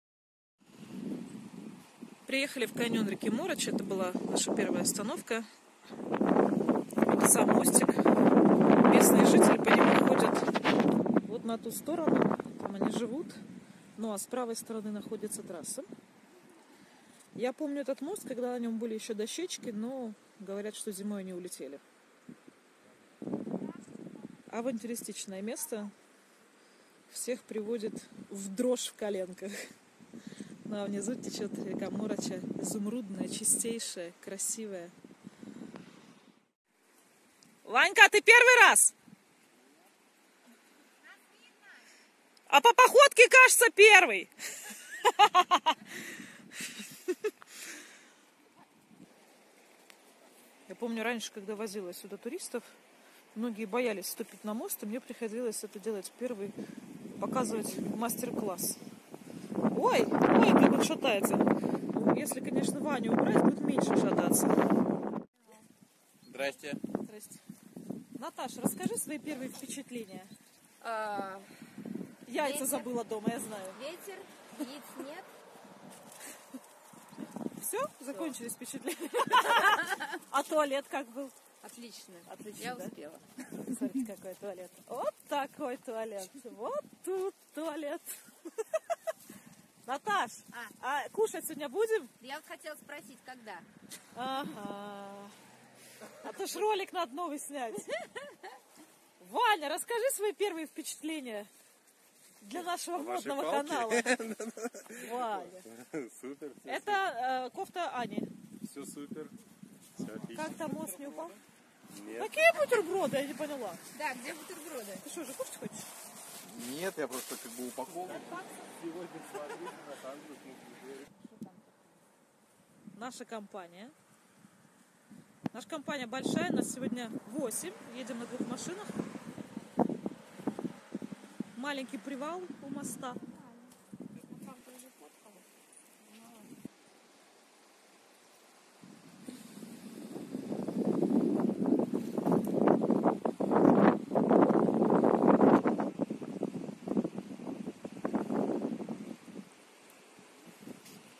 Каньон реки Морача, подвесной мост, Черногория 2017
Каньоны Черногории нельзя увидеть на автобусной экскурсии, их можно только прочувствовать, передвигаясь пешком и на авто в правильных местах. Жуткий и шатающийся мостик над рекой Морача приводит туристов в ужас. Наконец показала его друзьям.